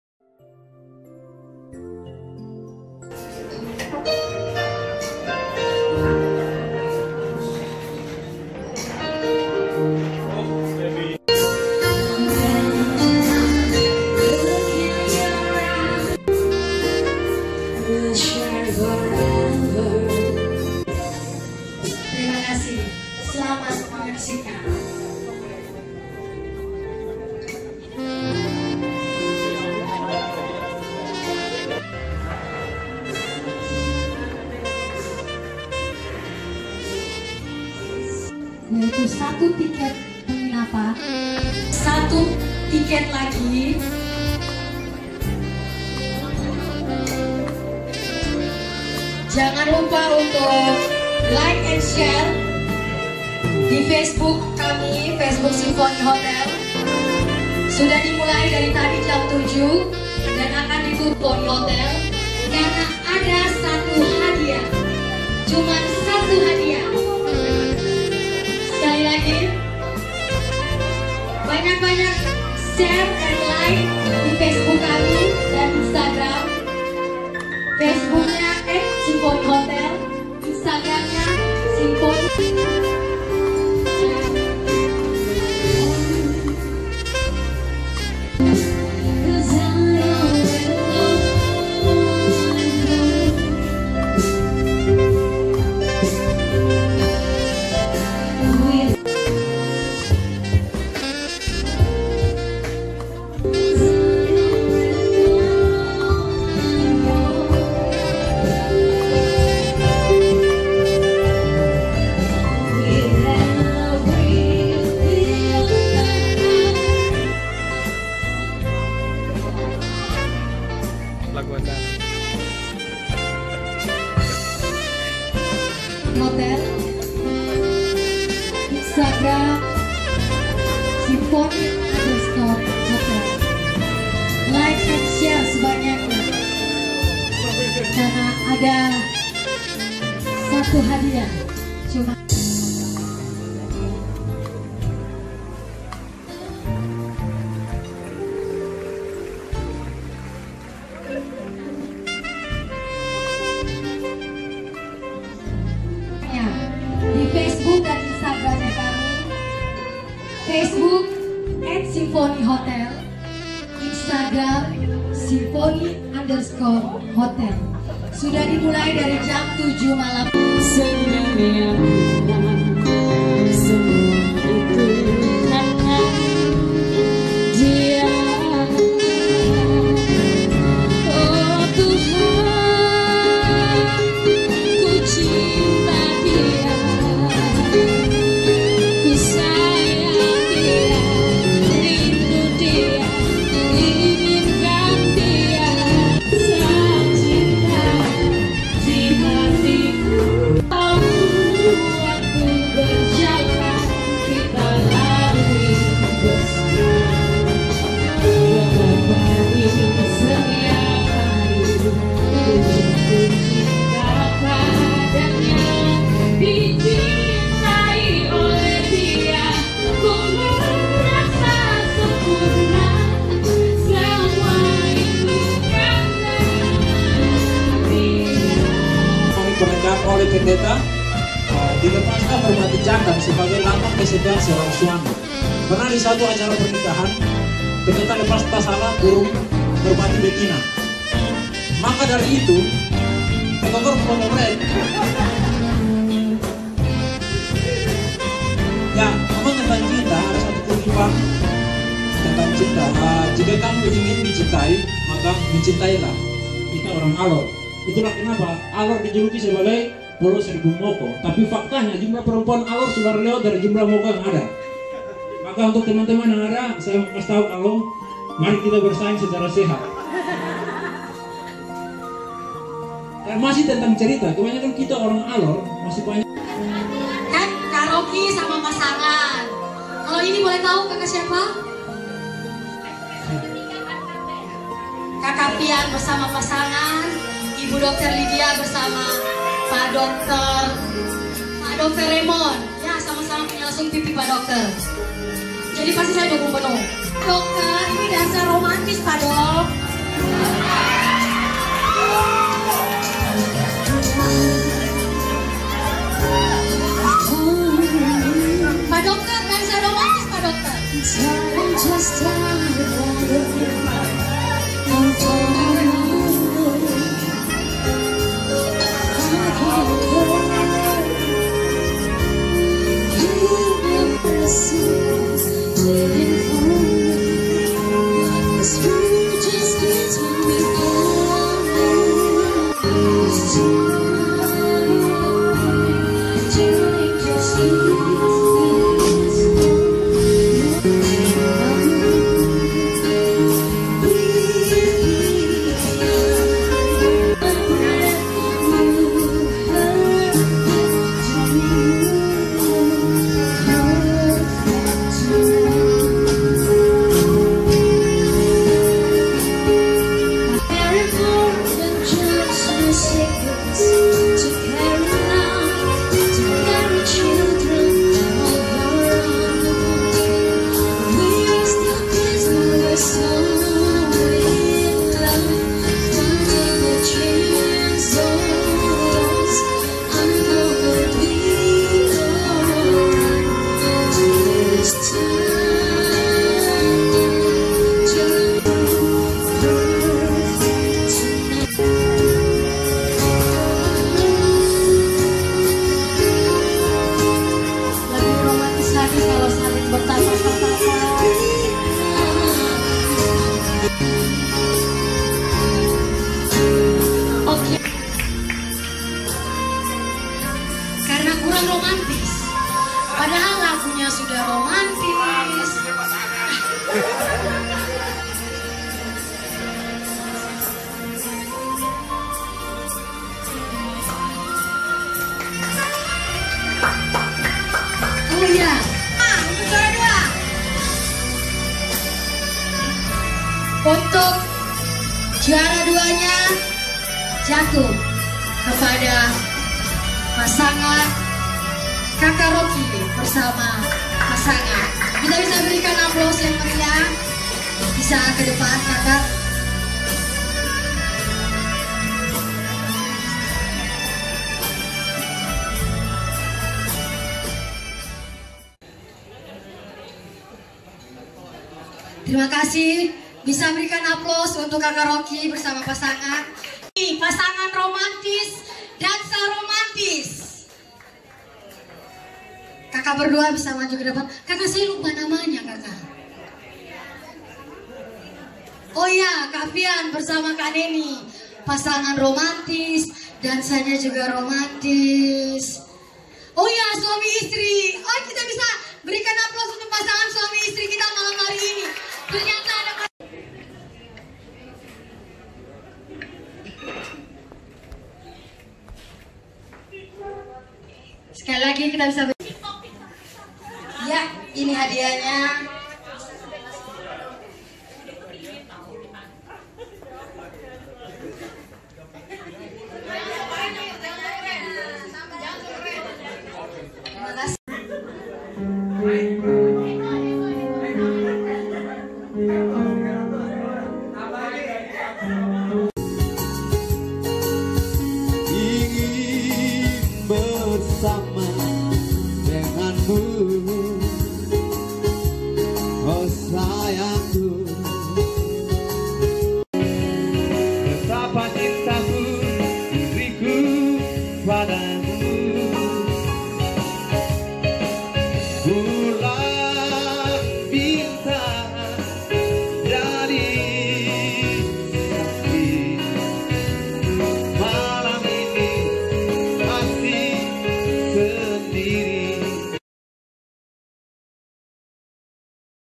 Romantic Valentine Dinner at Klasika Kafe 14 Februari 2020
Acara Romantic Valentine Dinner yang diadakan oleh Simfony Hotel Alor tepatnya di Klasika Kafe yang diikuti oleh 50 orang tamu berjalan dengan suasana hangat, dengan makan malam yang romantis, diiringi lagu-lagu penuh cinta, dilengkapi dengan sedikit hiburan Stand Up Comedy, dan kemeriahan lomba dansa dan kuis yang seru untuk tamu.
Nantikan acara-acara menarik berikutnya by Simfony Hotel Alor 😉